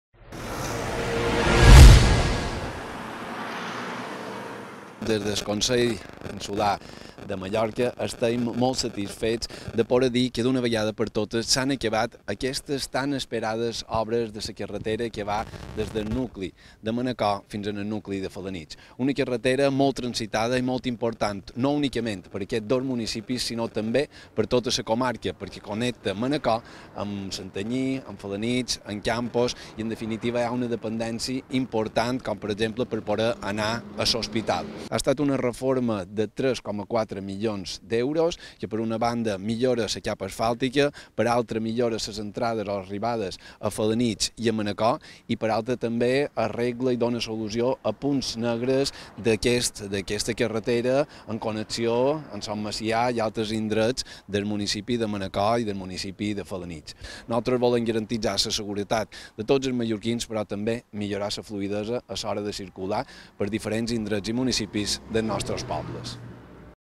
El Consell de Mallorca culmina la remodelació de la carretera de Manacor a Felanitx
El president del Consell de Mallorca, Llorenç Galmés, ha inaugurat avui les obres de remodelació de la carretera Ma-14, de Manacor a Felanitx. Una reforma que suposarà una millora important de la seguretat d’una via molt transitada entre les comarques de Migjorn i Llevant de Mallorca. Amb un pressupost de 3,4 milions d’euros, s’ha substituït tot el ferm al llarg de la carretera, molt degradat, per un d’alta adherència i s’han millorat la intersecció d’accés a Son Macià i les rotondes d’entrada i sortida de Felanitx i Manacor.